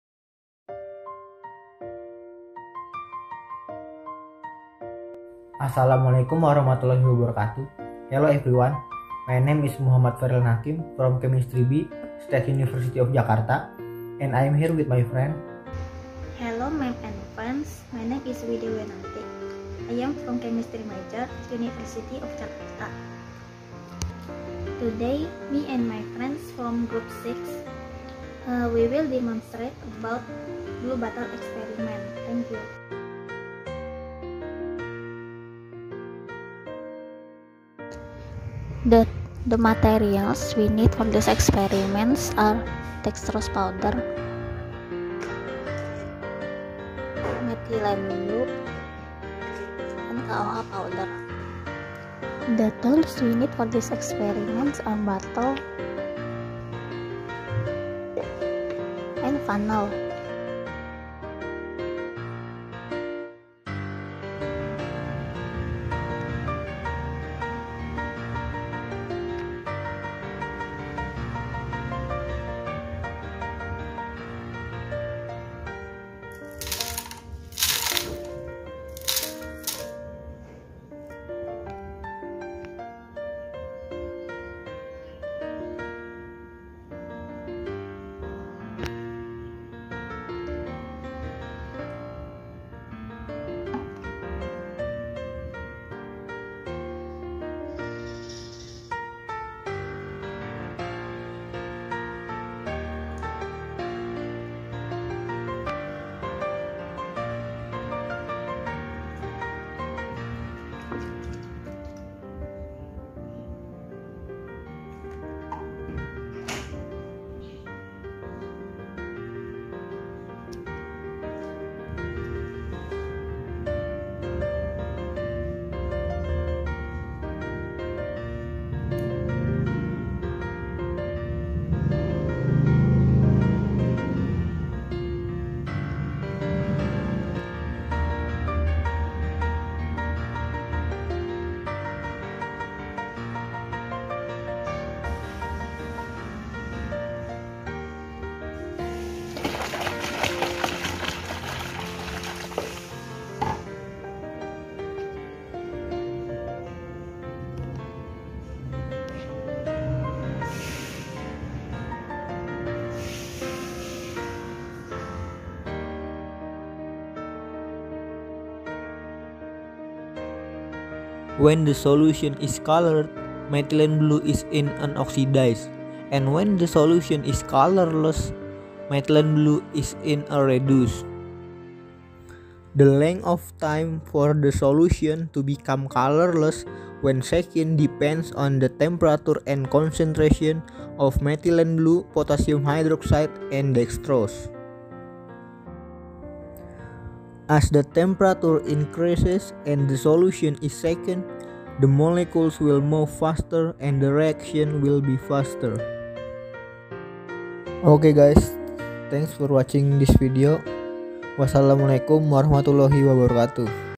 Basic Chemistry - Group 6 (Blue Bottle Experiment)
Hello Guys!!

We are from Group 6 Chemistry B 2021 State University of Jakarta

Members:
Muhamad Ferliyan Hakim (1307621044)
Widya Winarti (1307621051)

Subject : English for Chemistry
Lecturer : Irma Ratna Kartika, M.Sc.Tech.

In this video we will tell you about blue bottle experiment. Enjoy this video!!